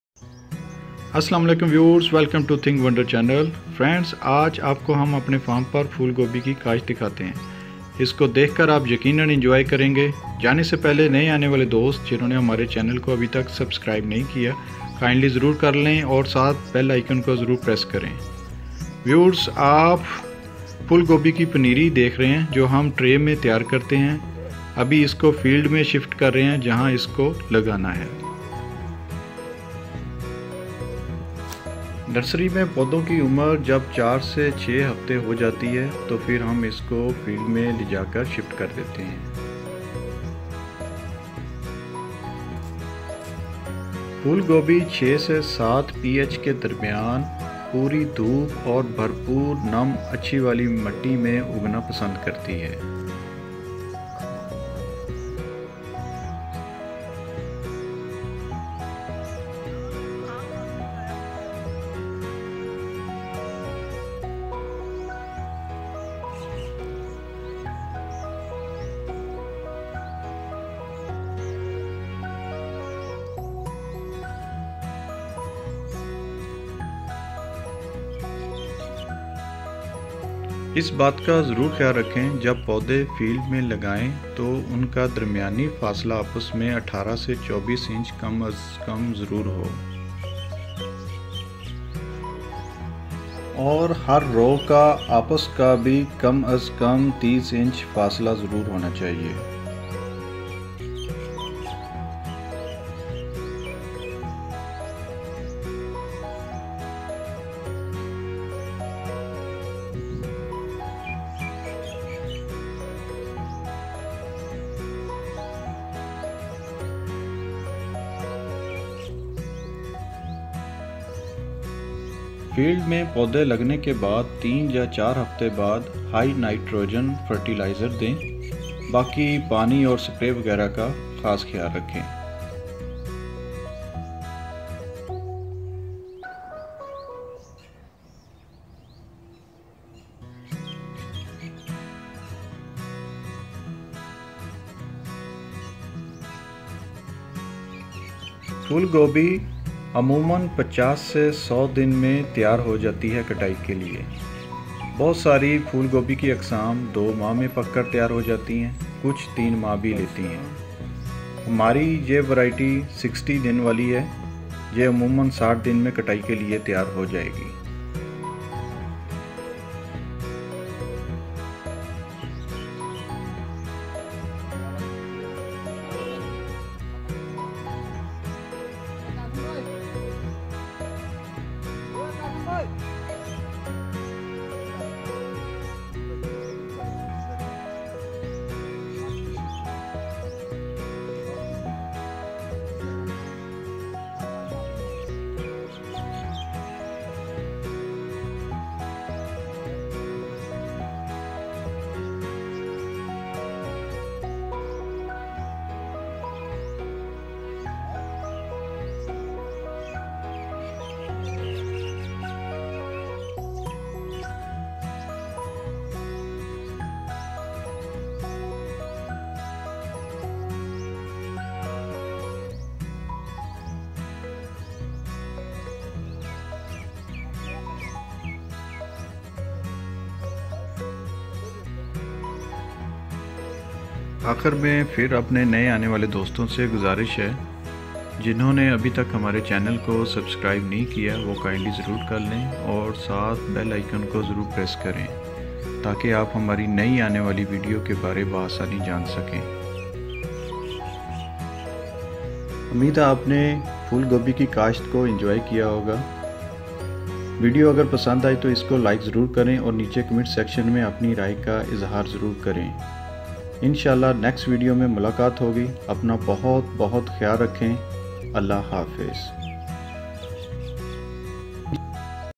Cauliflower How To Grow - Tips
In this video we are cultivating cauliflower crop at our farm.

Firstly we planted the cauliflower seeds in trays.

When cauliflower plants age became 4 to 6 weeks in our farm nursery.

Then we transplanted them into field. When planted in field. we arranged 18 inch distance between them and all rows have also 30 inch distance  between each other.

After 4 to 6 weeks give them high nitrogen fertiliser  and provide good care of water and pesticide.

cauliflower crop take mostly 50 to 100 for maturity.
our this variety will take 60 days.